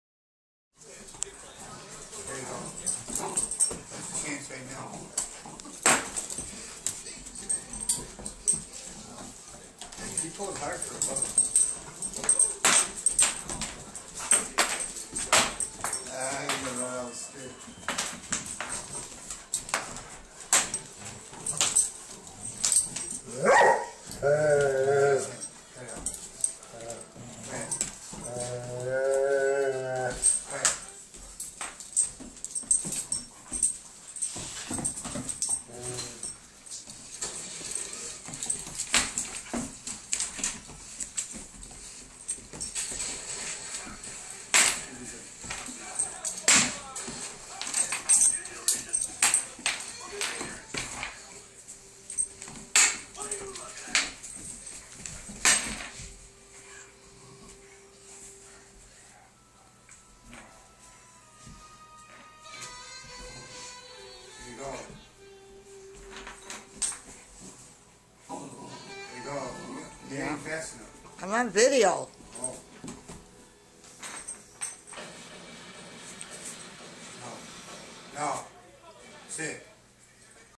doberman bella pulling leash.3gp
thought she had it made til momma put all her weight into it lol 2/12